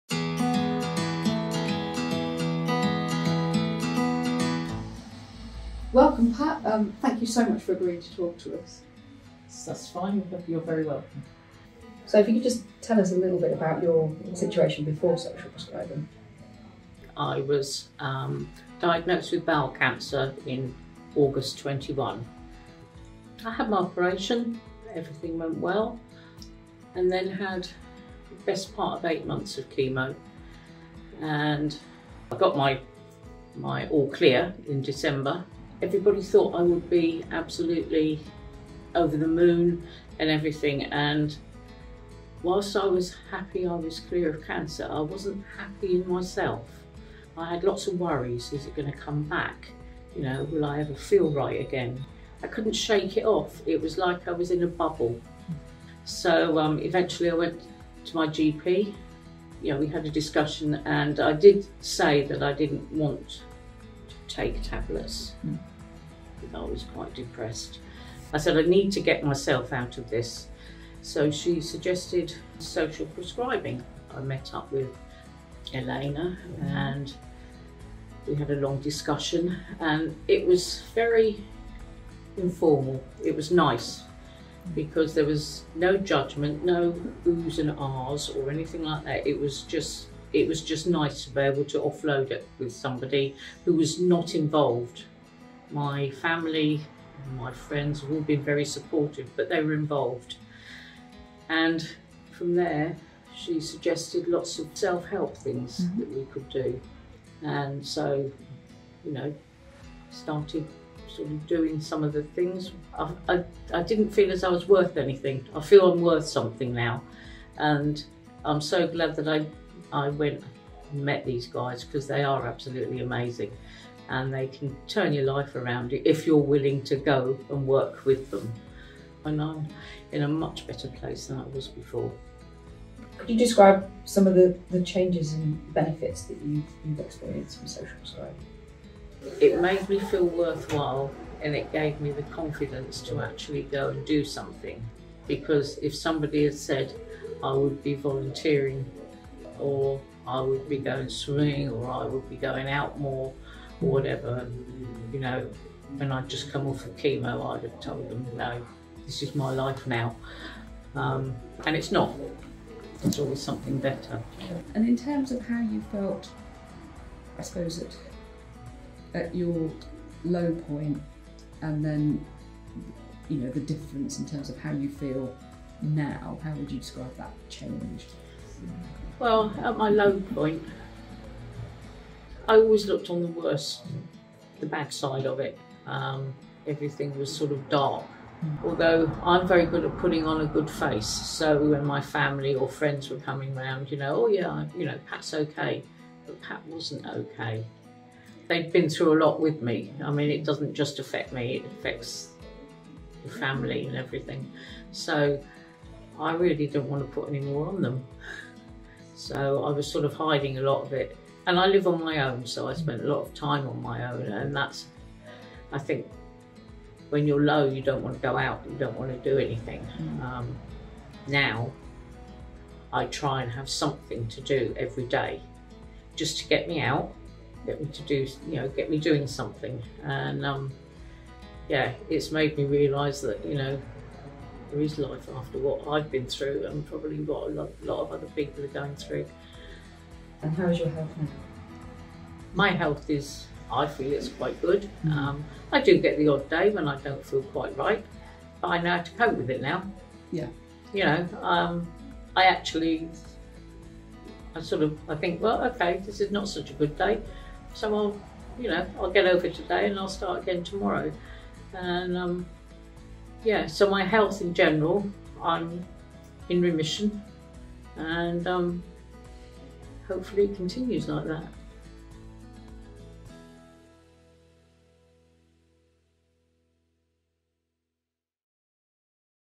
Social Prescribing diaries - Pat's story
Here’s Pat Bromley talking about how social prescribing has improved her quality of life following a cancer diagnosis.